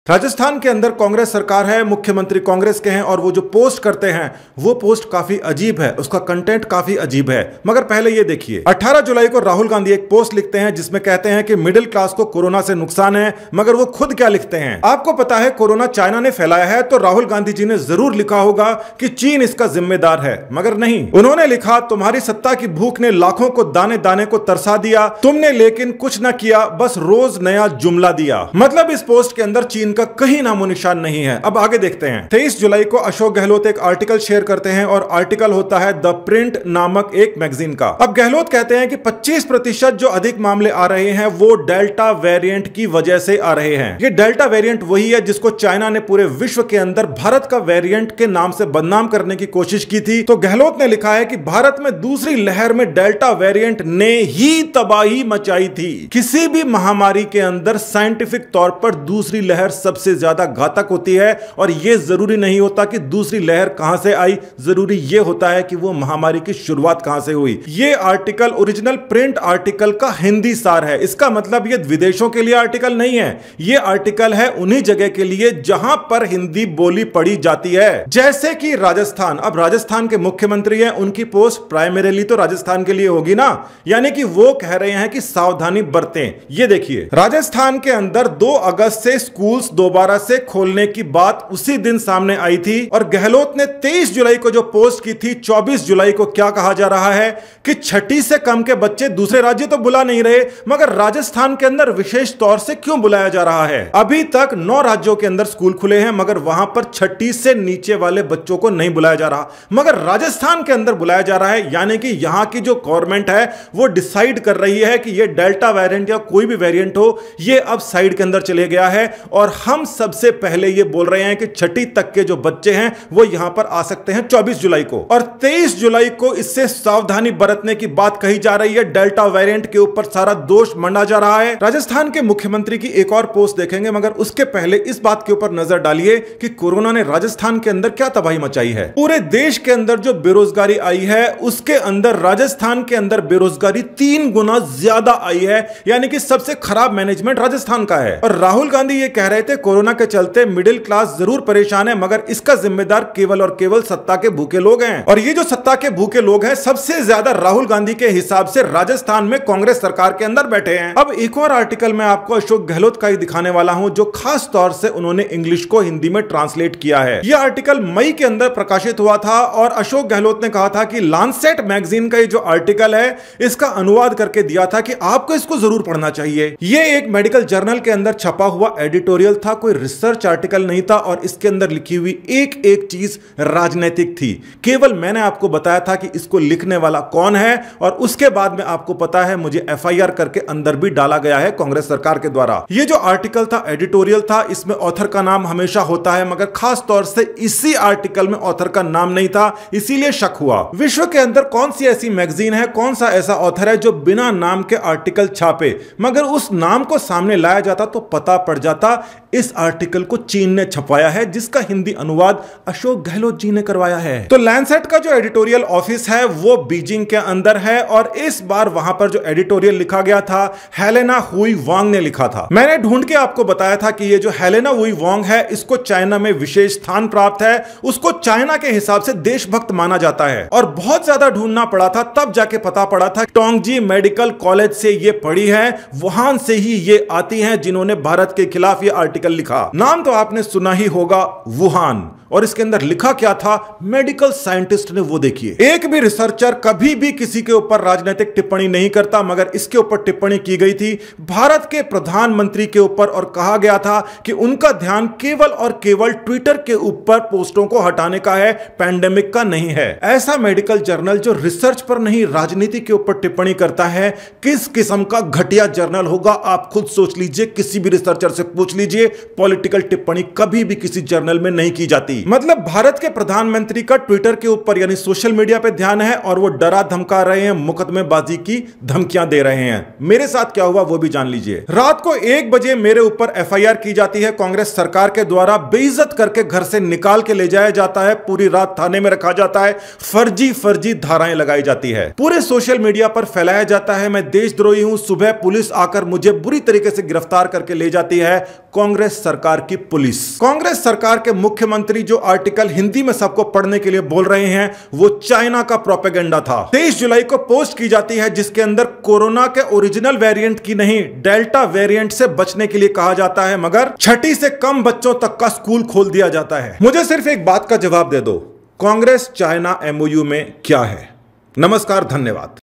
China and allies blaming delta variant to reduce damage on China image
एक दो नहीं कई सबूत जहाँ चीन के दोस्त उसको बचाने के लिए किस हद तक जा रहे हैं वो देखो | संयोग है या साजिश आप खुद फैसला करो